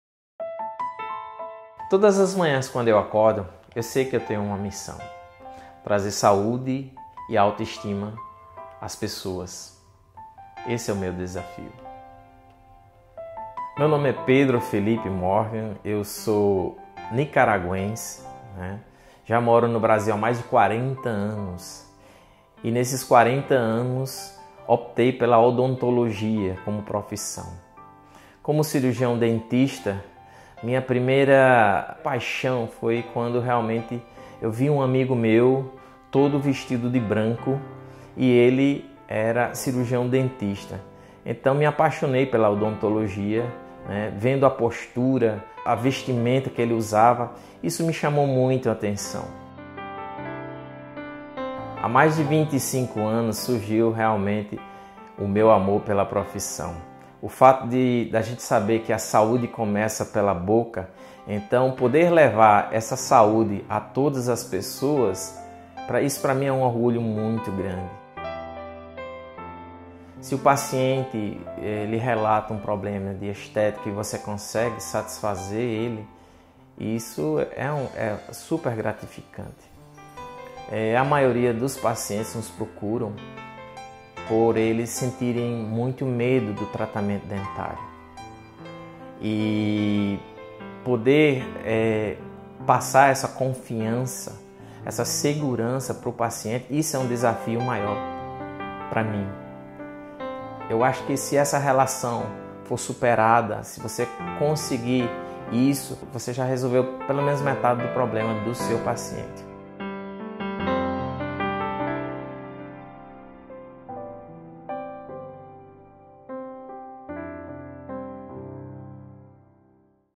Dr. Pedro Morgan
Nesse vídeo relato meu amor pela odontologia e minha relação com os pacientes.